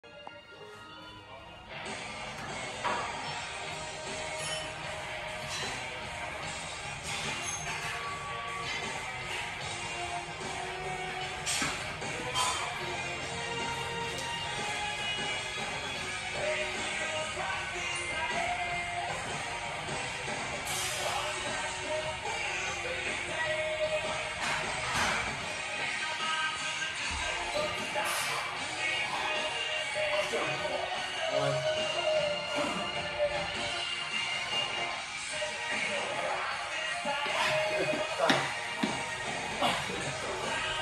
Safety Bar Chaotic Box Squats 📦 112 kg x 2
Squatting with an SSB (safety squat bar) with hanging plates to a 16 inch box for hit the stabilizers. Went for 5 but got 2 😄